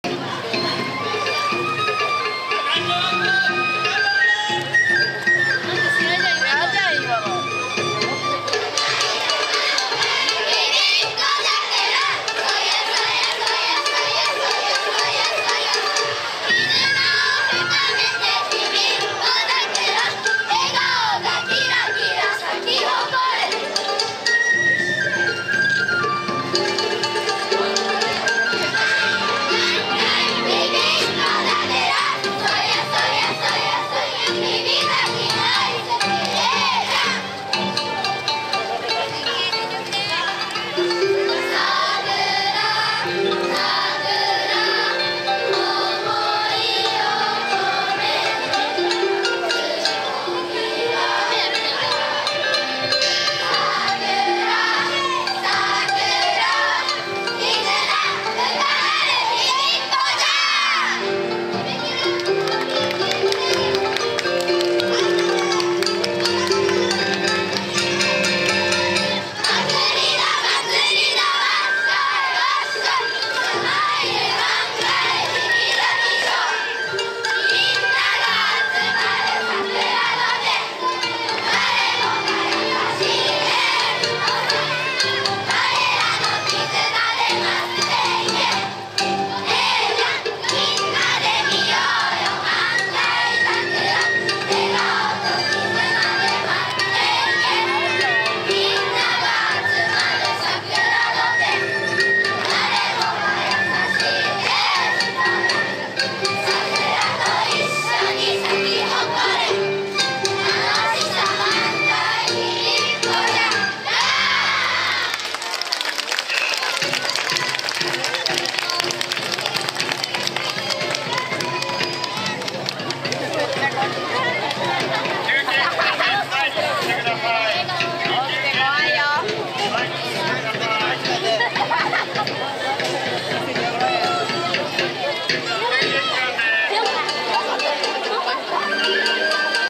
第８１回尾道みなと祭【第２２回ええじゃんＳＡＮＳＡ・がり・踊りコンテスト・２日目】㉑きずな深めて笑顔満開日比っ子桜
広島県尾道市
海岸通り
第８１回尾道みなと祭
２日目
第２２回ええじゃんＳＡＮＳＡ・がり
２日目
踊りコンテスト
幼児・小学生・中学生部門
小学生部門
「きずな深めて笑顔満開日比っ子桜（尾道市立日比崎小学校）」さん
第１エリア・スタート・１１：５８
第８エリア・審査・１２：２２
ステージ披露・１２：５０
出場回数・２２回
出場人数・９５名
㊟事前発表の資料なので実際と異なる場合あり
令和６年４月２１日（日）海岸通り（広島県尾道市土堂１丁目）